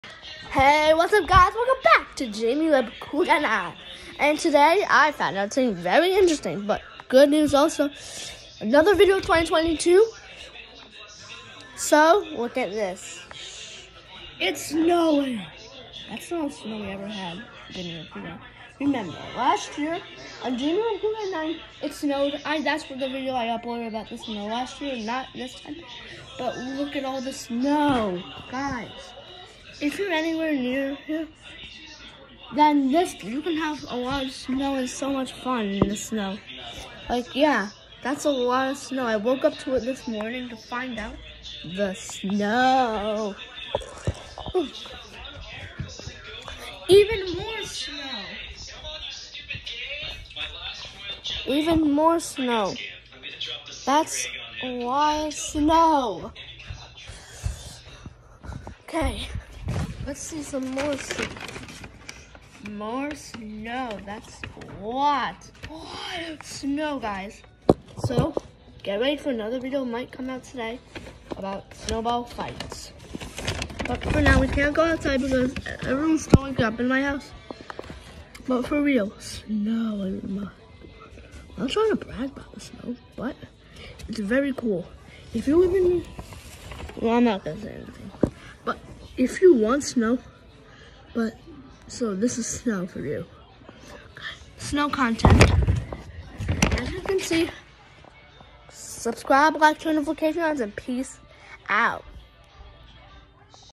A lot of snow
Last year I uploaded a video about the snow here’s another one but much better also this is my 2nd video of 2022